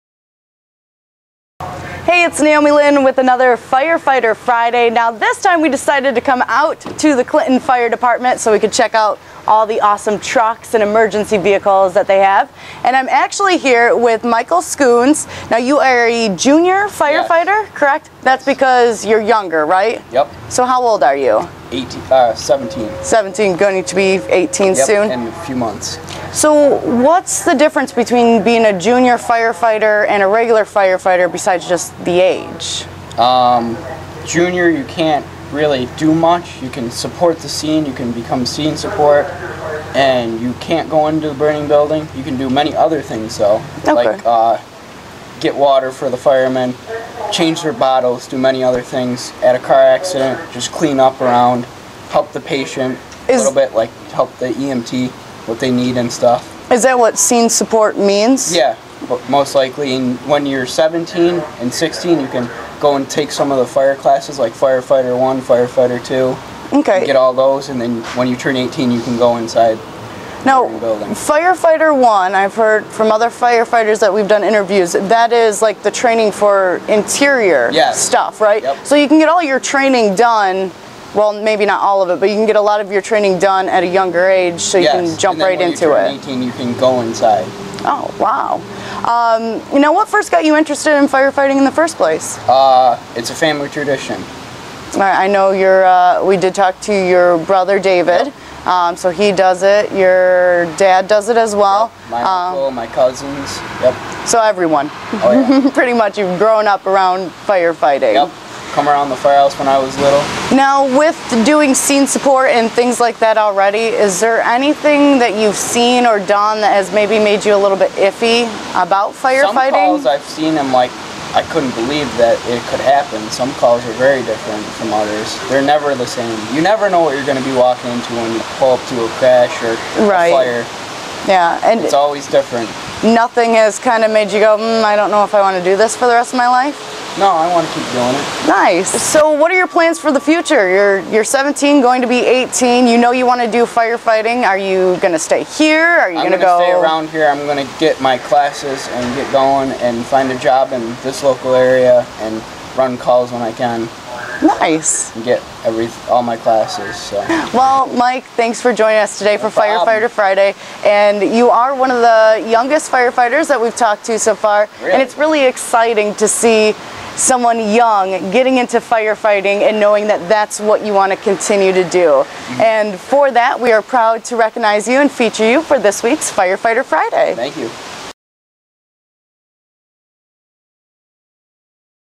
Young Adult Knows Firefighting is What He's Meant to Do
For this week's 'Firefighter Friday' we talk to junior firefighter Michael Scoones. He may be young, but he's passionate and dedicated to being a firefighter. He also explains what it means to be a junior firefighter.